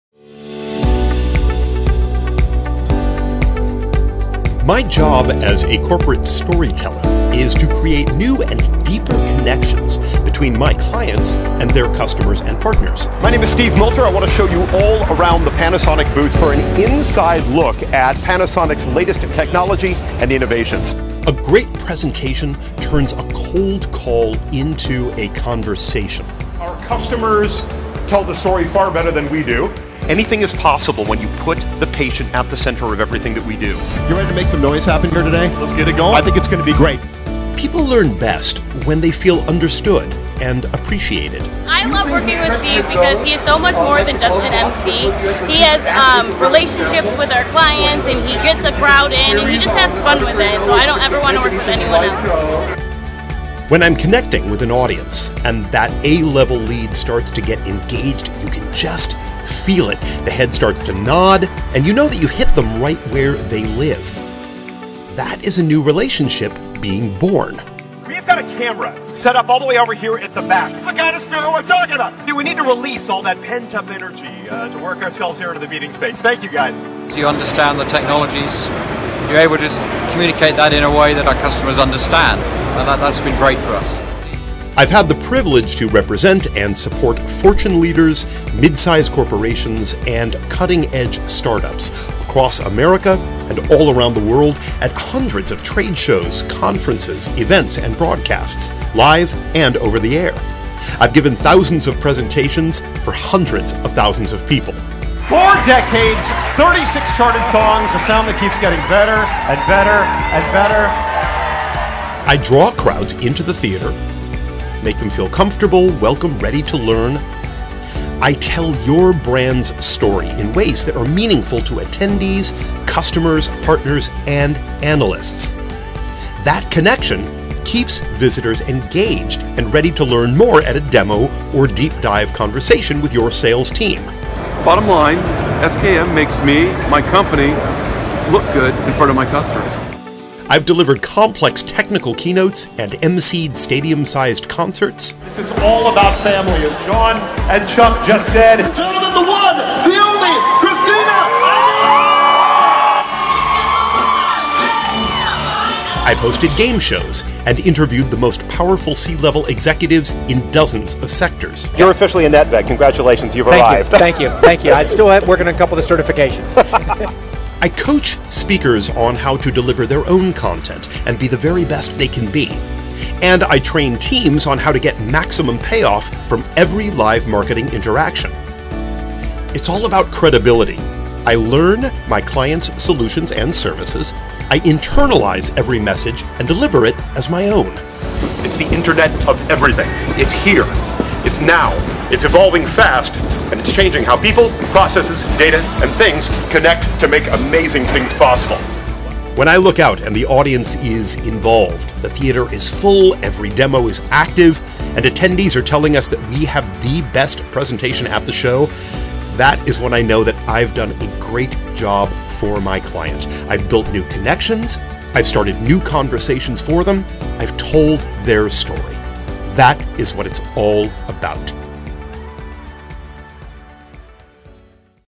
Steve Multer. Corporate Storytelling.
For over 20 years and more than 500 events, broadcasts, and trade shows, Steve Multer has been the storytelling face and voice for many of the largest brands in the world. Steve is a trusted trade show presenter, event emcee, broadcast host, featured speaker, corporate trainer, and C-Level keynote coach for Fortune, Midsize, and Startup companies across America, Europe, and Asia.

Steve creates connections between corporations and customers, increases awareness for his clients and their messaging impact through expert event, conference, and trade show representation including presentation, hosting, creative content development, speaker training, and event staffing.